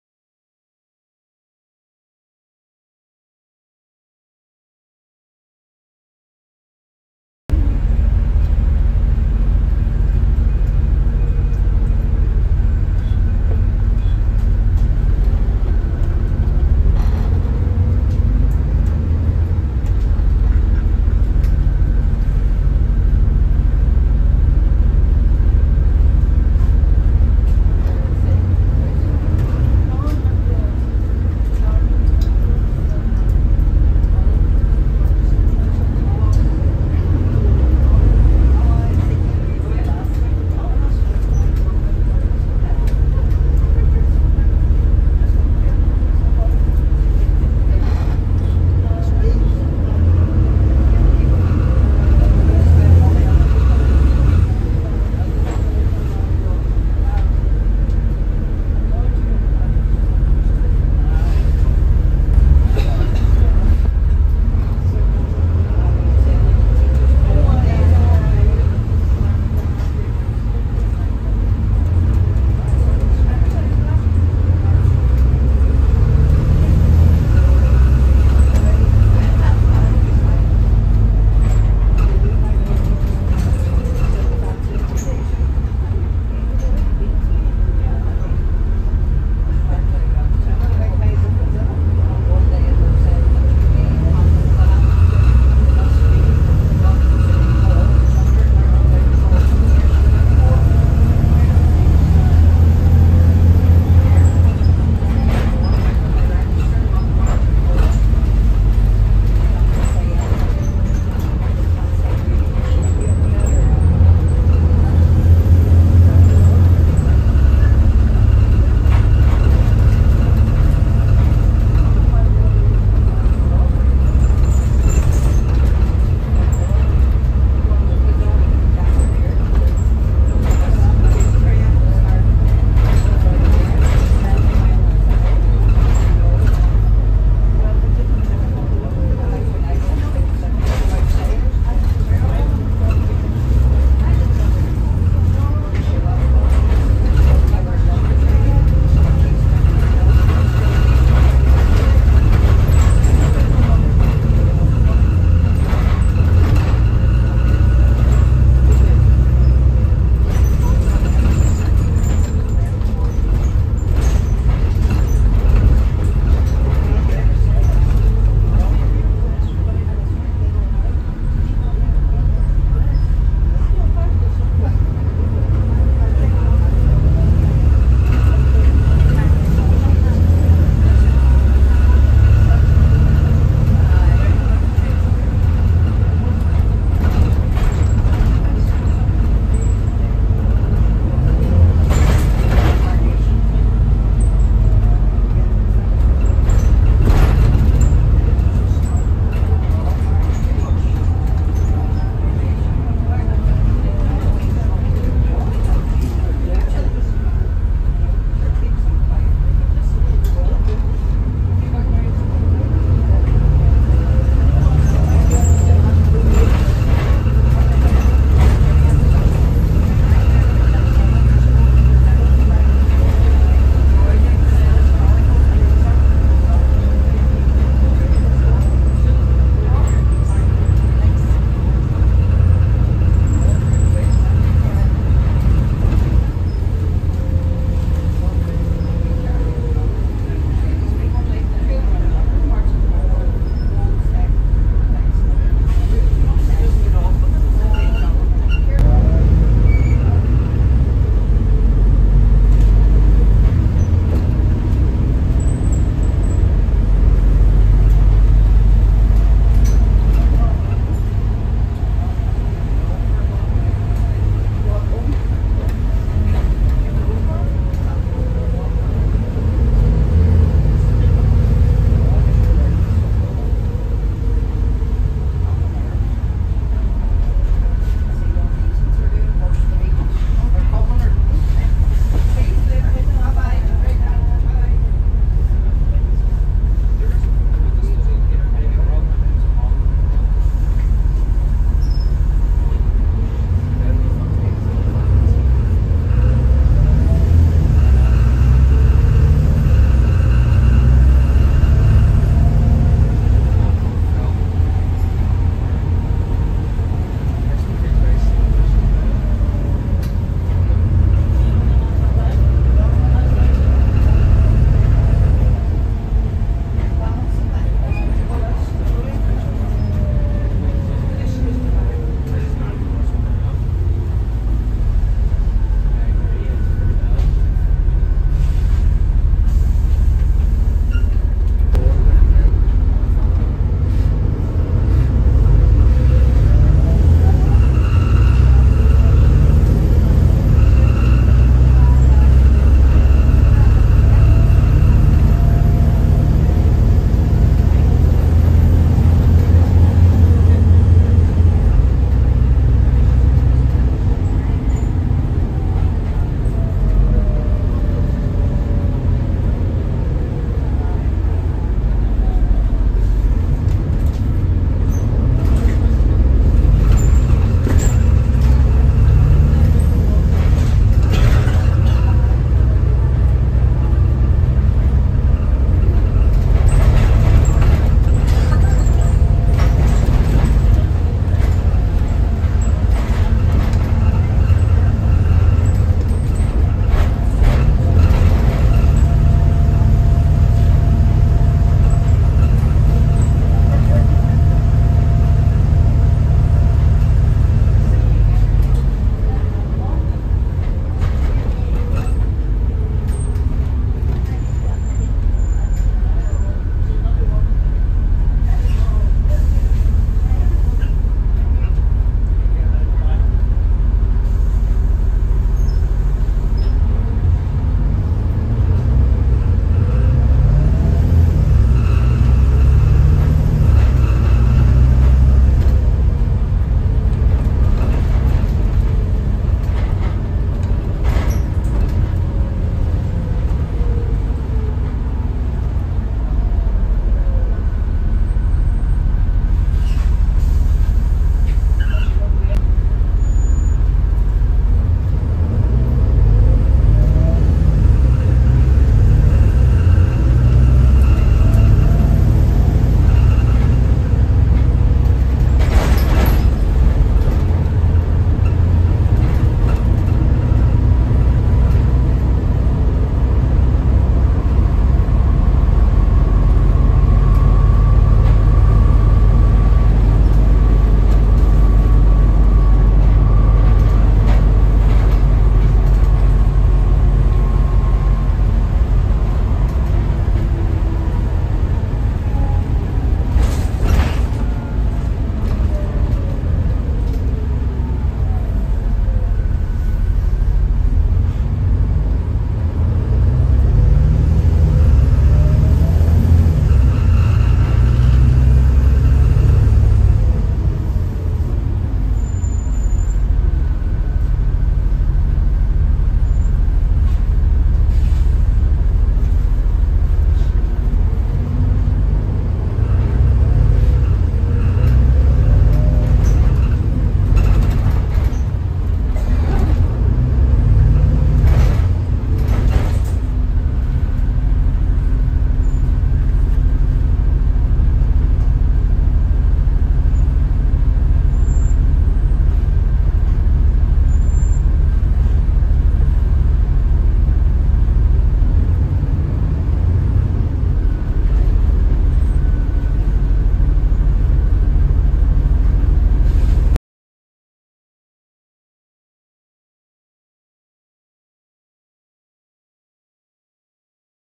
Translink Ulsterbus Volvo B7TL ALX 400 2321 (Route 384c)
Small spin on one of Omagh B7TL’s 2321 on Omagh Town Service 384c.

It is believed that the last of Omagh’s ALXs could be gone by June with Metro diesel replacements due soon for Omagh.